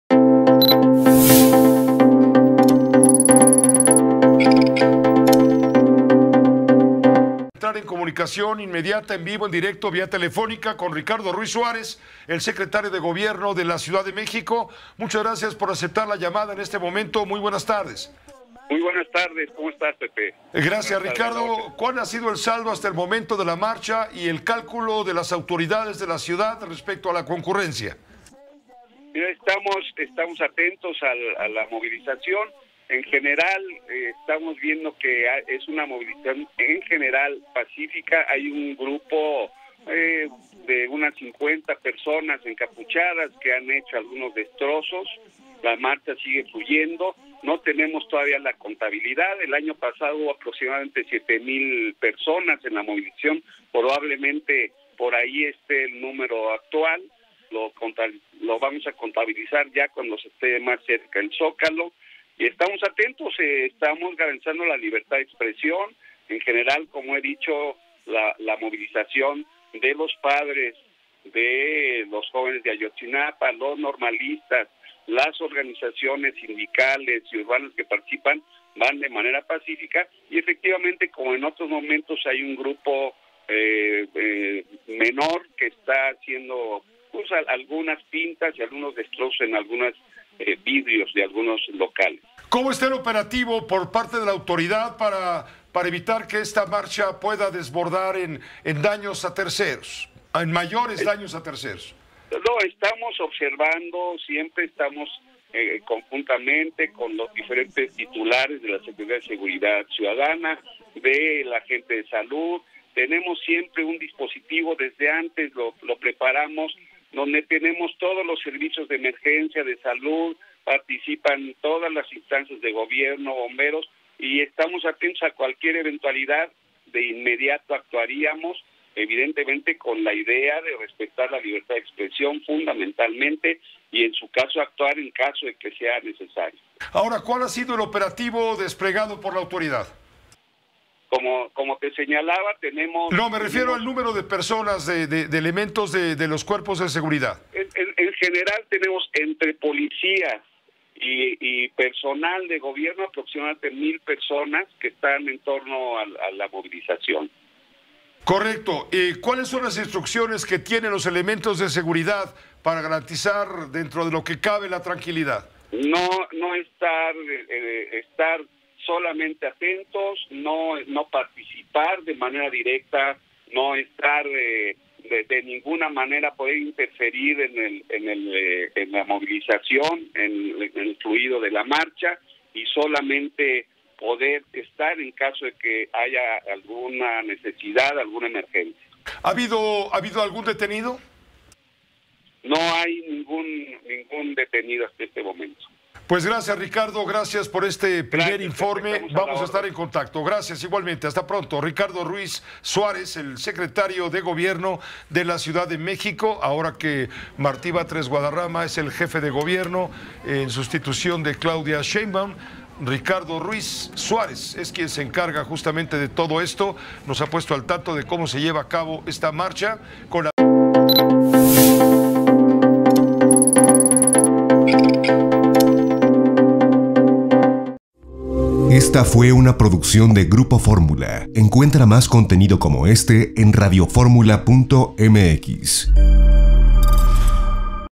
Suman 4 mil personas en la marcha de Ayotzinapa: Ricardo Ruiz
Ricardo Ruiz Suárez, secretario de gobierno de la CDMX, en entrevista con José Cárdenas, comentó con respecto al último reporte de la Secretaría de Seguridad Ciudadana de la Ciudad de México (SSC-CDMX) ya suman más de 4 mil personas ya se encuentran en esta marcha. Todas las calles y avenidas que cruzan Paseo de la Reforma hasta el Centro Histórico ya están cerradas. Hay 800 elementos de Tránsito y Seguridad Pública para salvaguardar la integridad de las personas durante la marcha. Llegaron grupos de jóvenes encapuchados con palos y tubos, registrándose destrozos, fueron confrontados por los manifestantes para detener los disturbios, hasta el momento no se reportan personas detenidas.